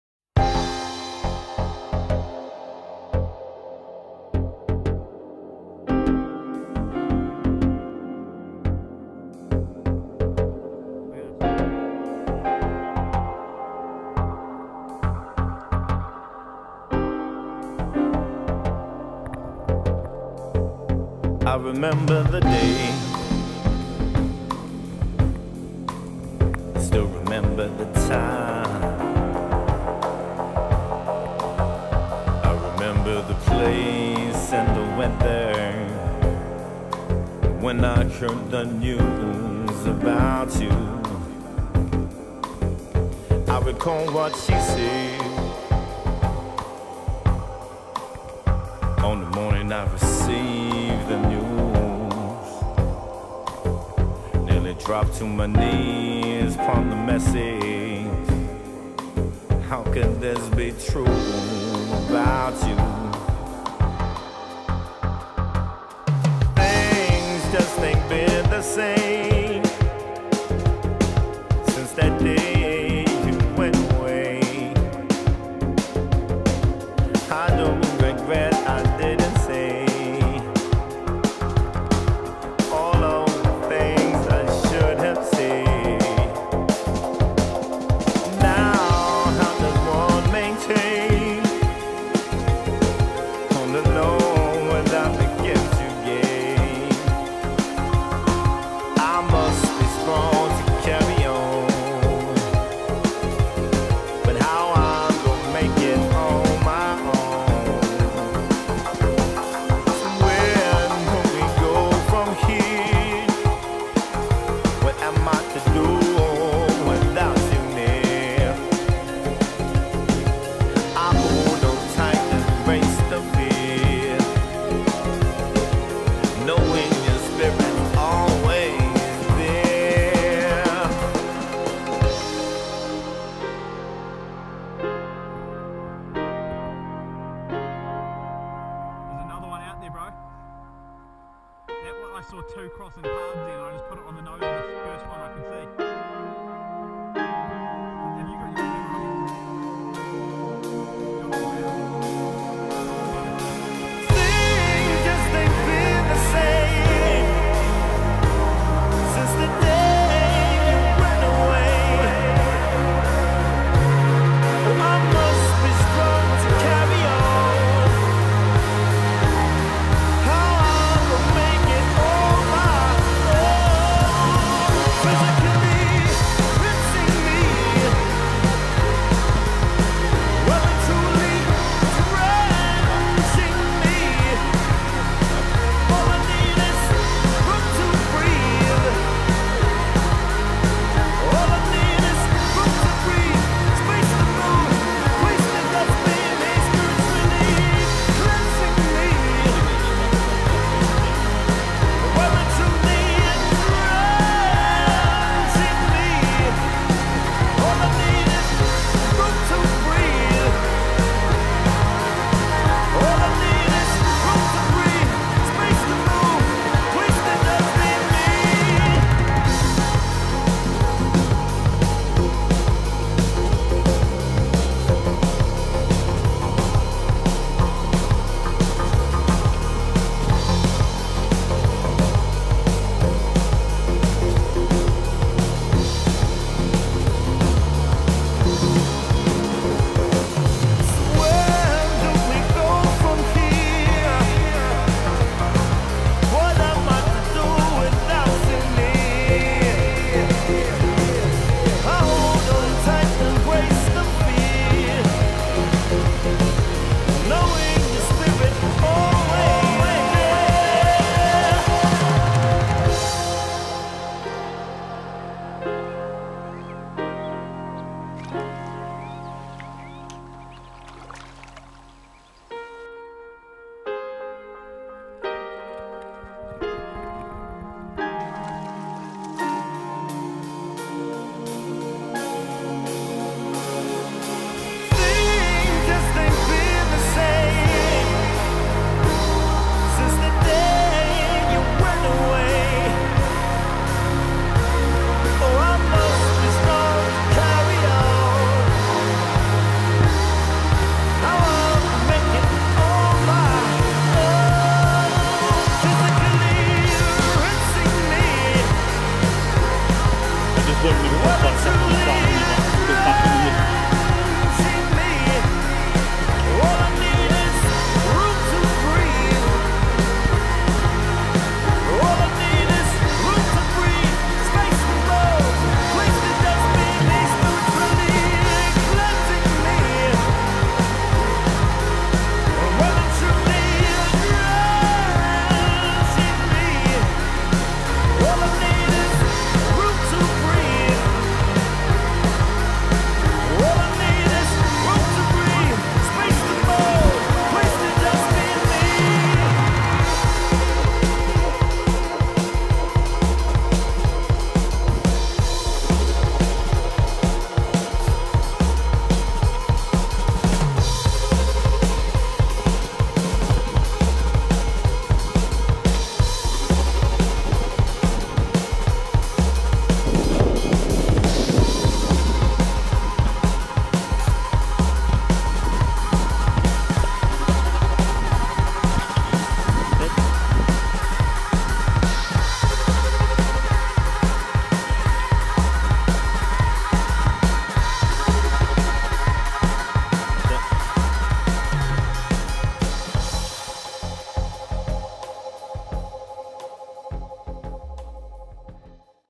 Pearson when the fish are on.wmv
A day at lake Pearson with light gear and lots of winter dry fly action.